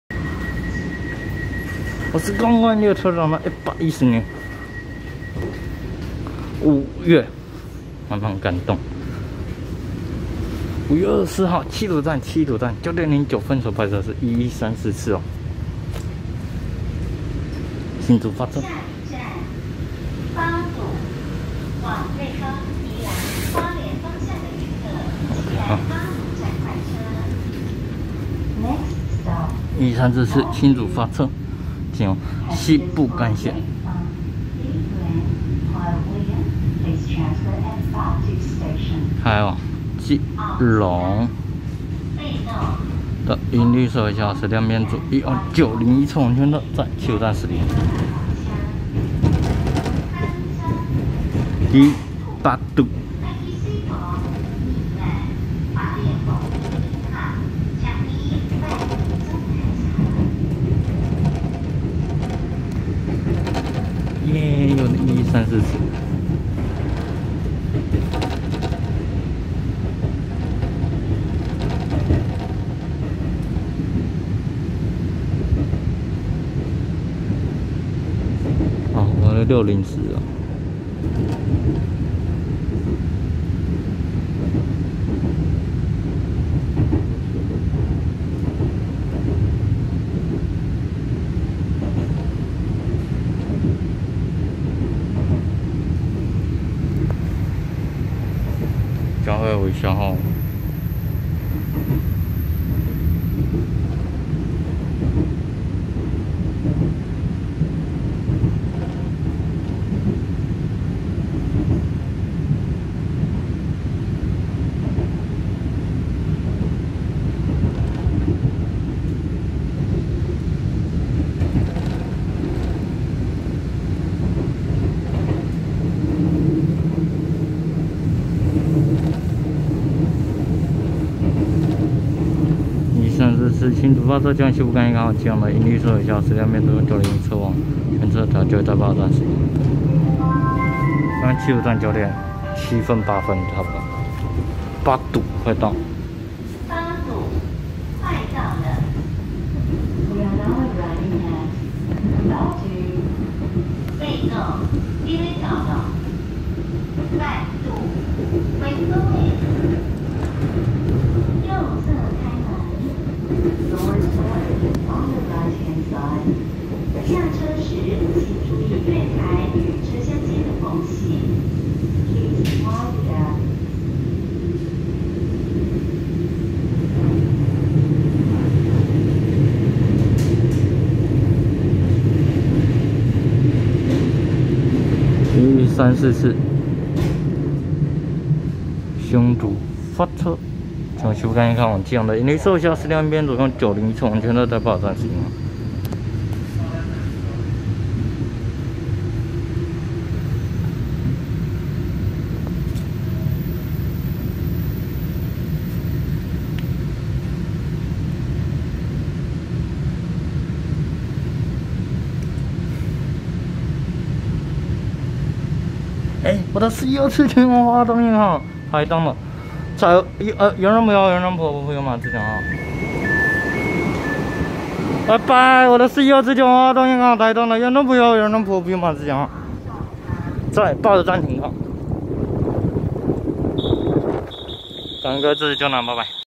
1134次新竹發車經由西部幹線開往基隆的銀綠色微笑號EMU900型十輛一編組EMU901車王區間車，和412次經由花東線開往台東的TEMU2036+TEMU2035原裝普悠猴原裝普猴普悠瑪自強號
110/5/24七堵站9:09分拍攝1134次新竹發車經由西部幹線開往基隆的銀綠色微笑號EMU900型十輛一編組EMU901車王區間車，和412次經由花東線開往台東的TEMU2036+TEMU2035原裝普悠猴原裝普猴普悠瑪自強號